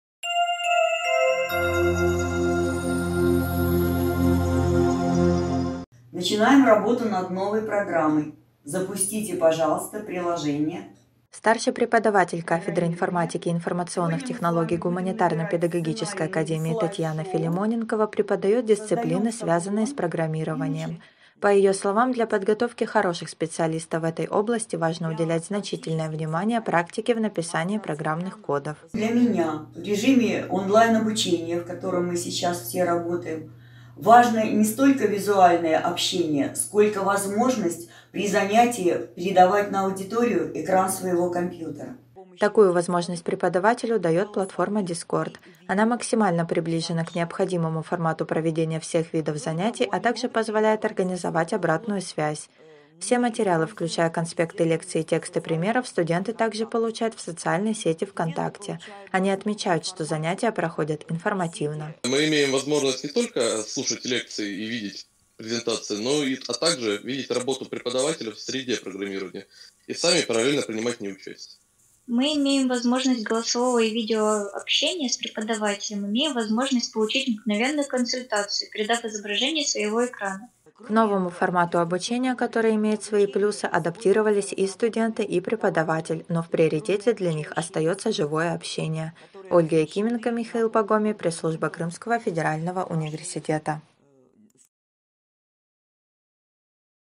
Дистанционное обучение – Татьяна Филимоненкова, ст. преподаватель ГПА КФУ им. В.И. Вернадского.
Старший преподаватель кафедры информатики и информационных технологий Гуманитарно-педагогической академии Татьяна Филимоненкова преподает дисциплины, связанные с программированием.

The Online Study Mode - Tatiana Filimonenkova, The Senior Lecturer of the Humanitarian and Pedagogical Academy of the Crimean Federal University

The Senior Lecturer of the Department of Information Science and Information Technologies of the Humanitarian and Pedagogical Academy Tatiana Filimonenkova teaches the subjects connected with programming. She holds classes on the Discord platform which is as close as possible to the necessary format of all types of classes, as well as allows to organize feedback, and also the social network "VKontakte."